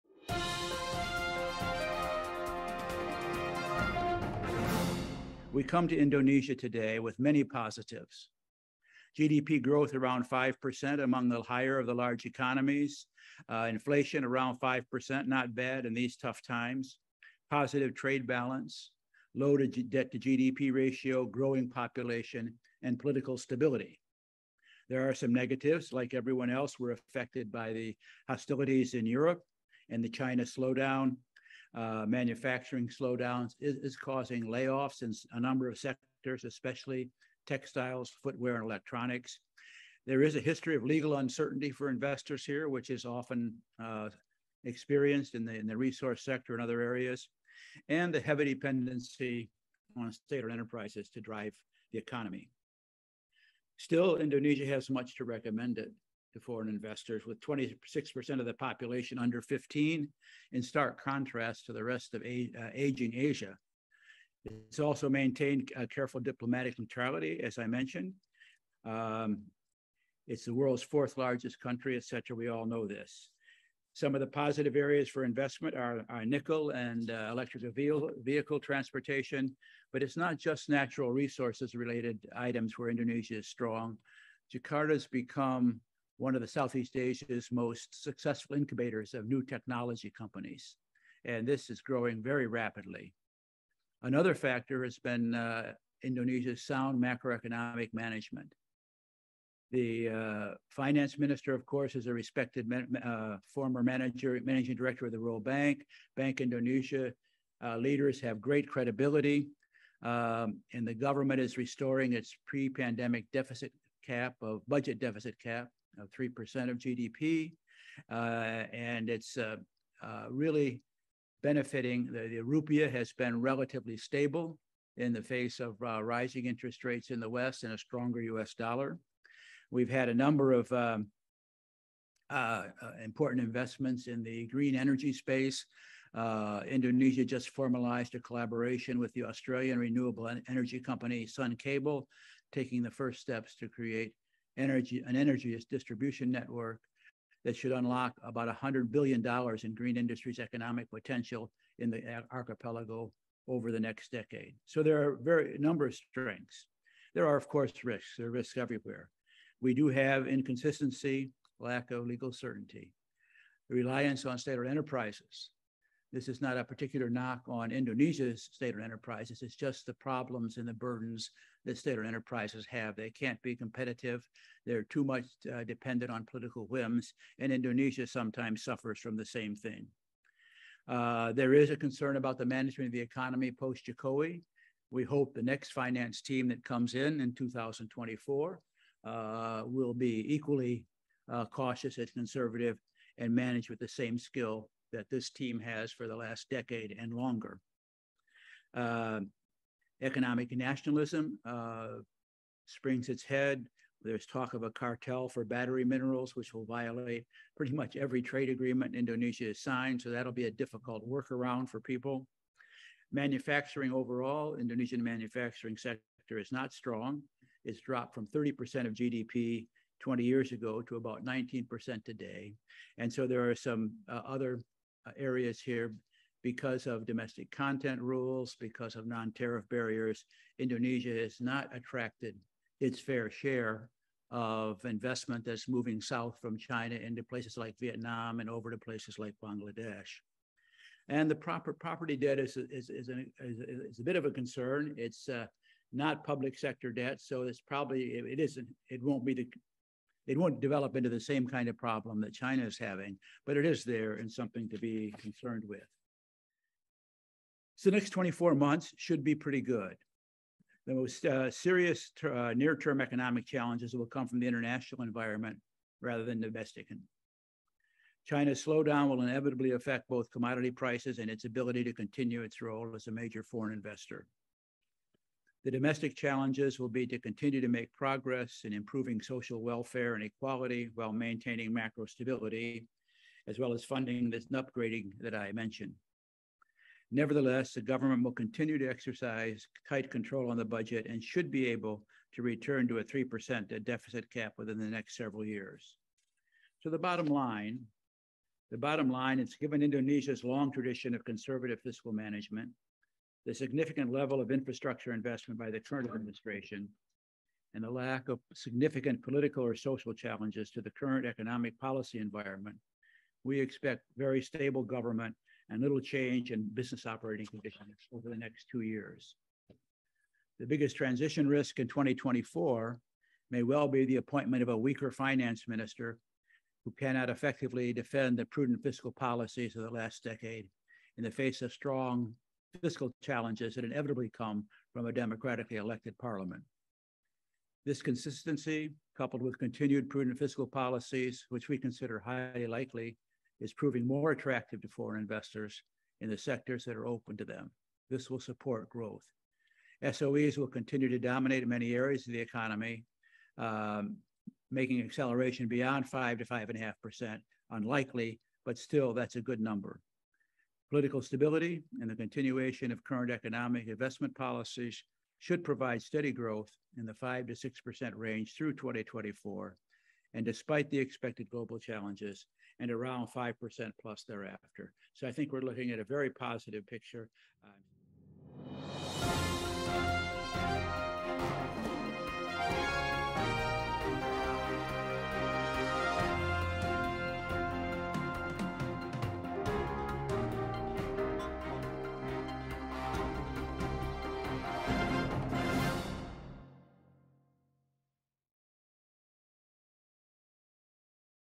James Castle on Economic Update, in conversation with Sachin V Gopalan
Indonesia Economic Forum 2022: James Castle, Chairman of CastleAsia, In conversation with Sachin V Gopalan, for an Economic Update, at the 9th Indonesia Economic Forum